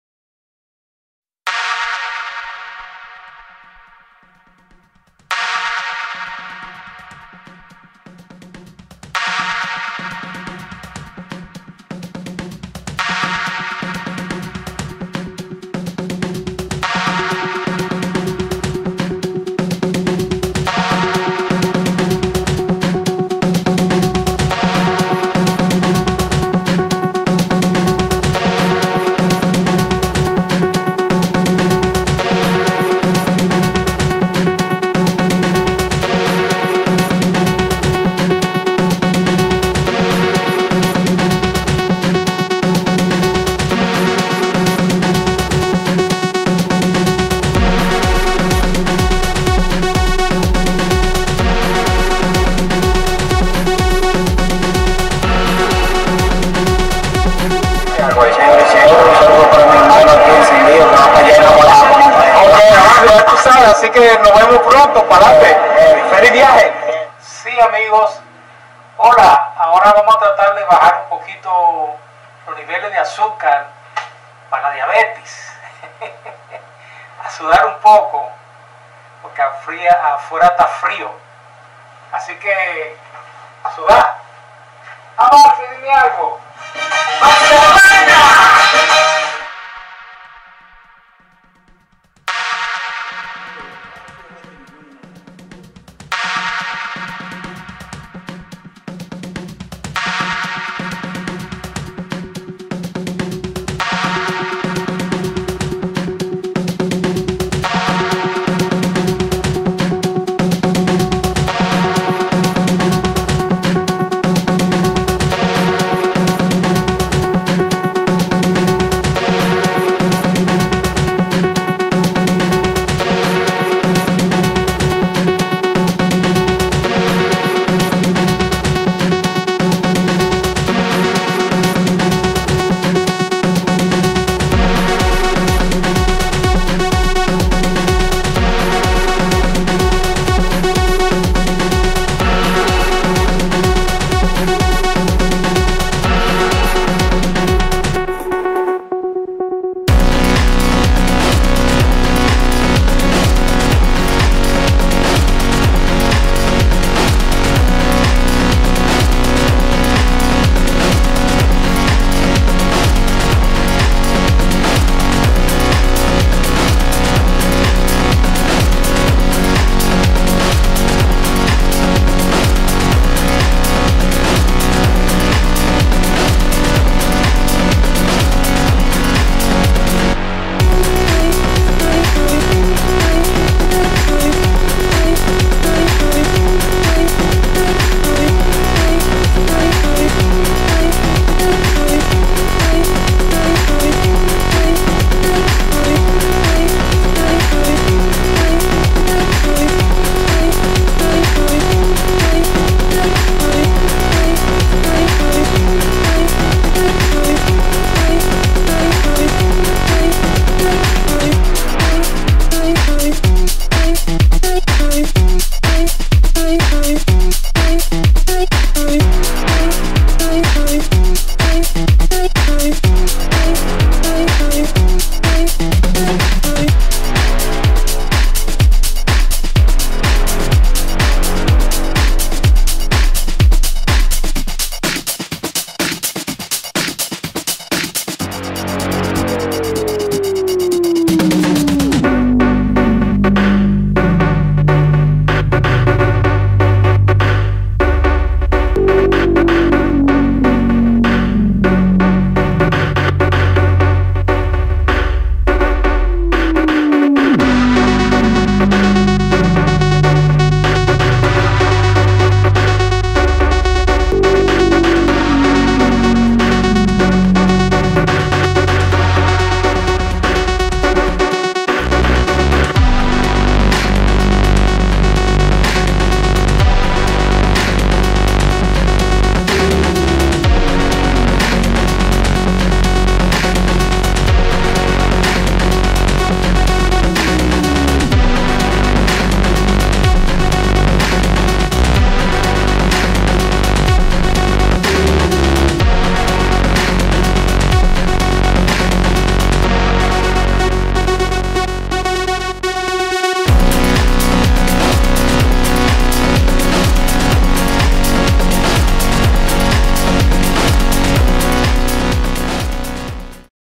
" UN DIA EN LA VIDA DE RAPHY HEREDIA 2 da PARTE "
CONTINUACION VIDEO " UN DIA EN LA VIDA DE RAPHY HEREDIA " SUS INTIMIDADES EN SU BATICUEVA .